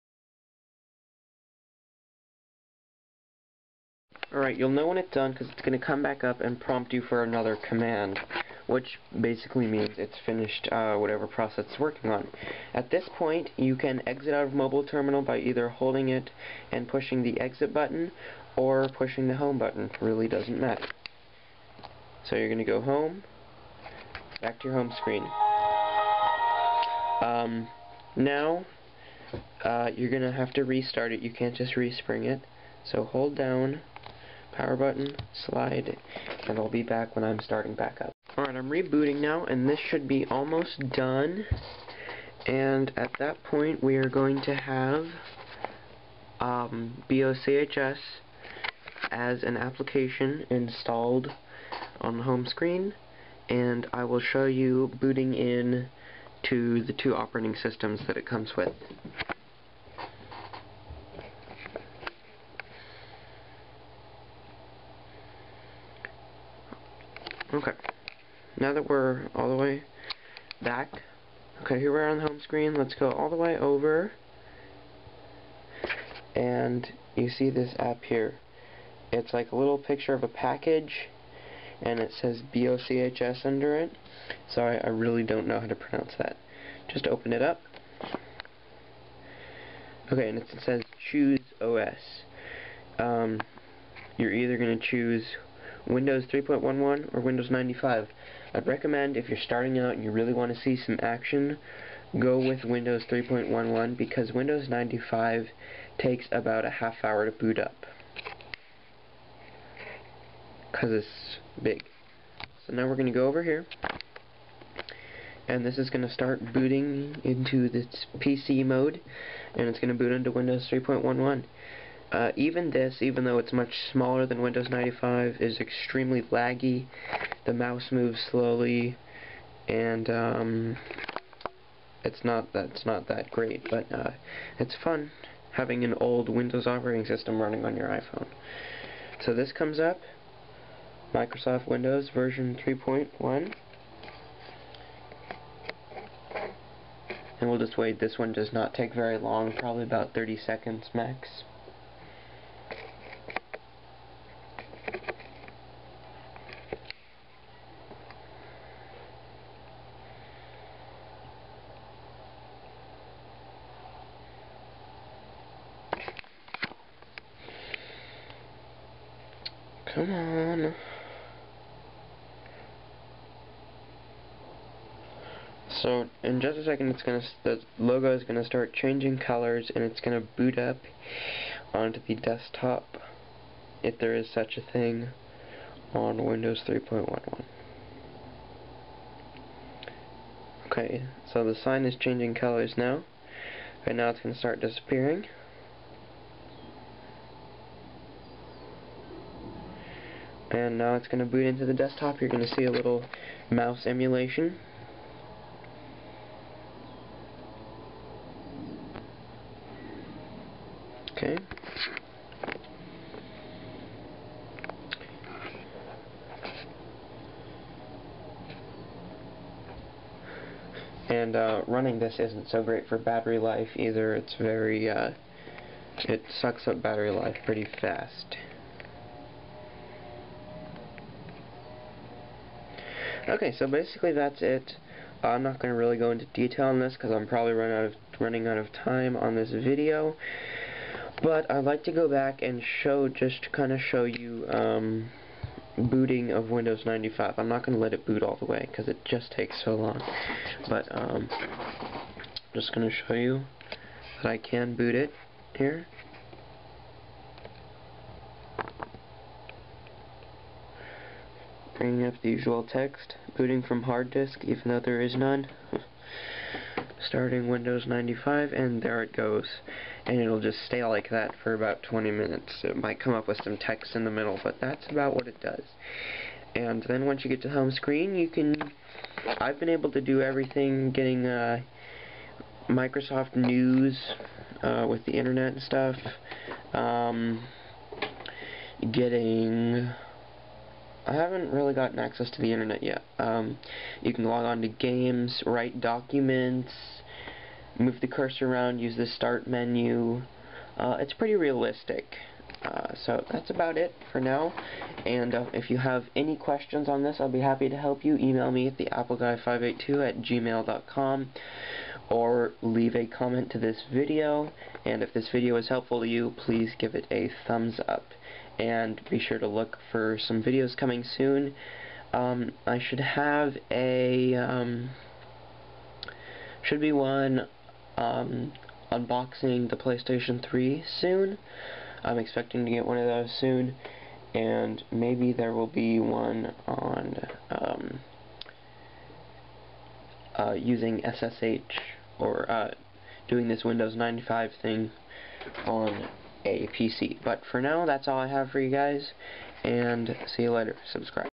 Installing windows 95 on iPhone Pt 2
Here is part two of installing windows 95 on the iphone. Here is a link two part one.